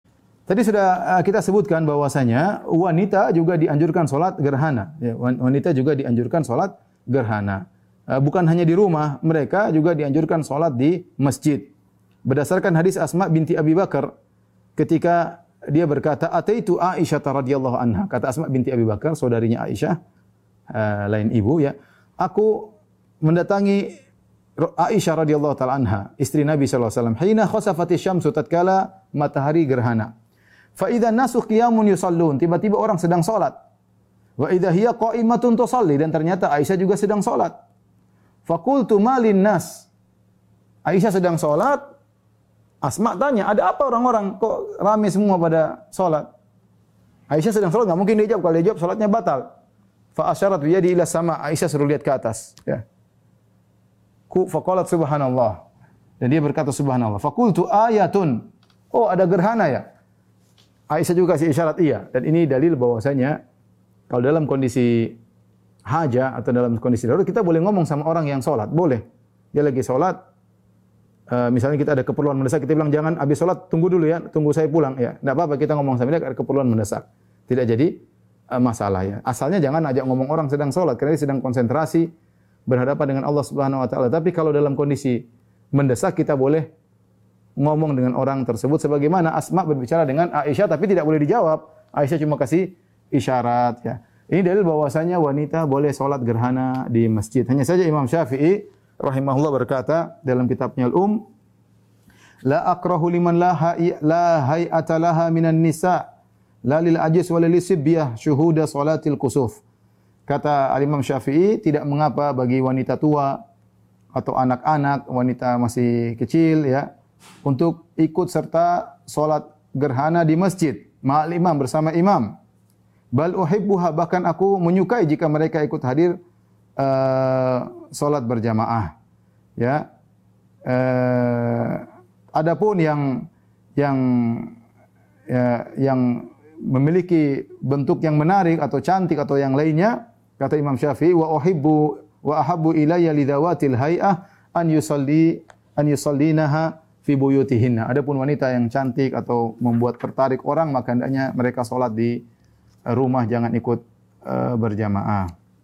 Wanita Juga Dianjurkan Sholat Gerhana Baik Sendiri maupun Berjama'ah
📝 Judul kajian: Syarah Kitab Bekal Sholat #7 - Sholat-Sholat Sunnah 2
🎙️ Penceramah: Ustadz Dr. Firanda Andirja, Lc., M.A. حفظه الله تعالى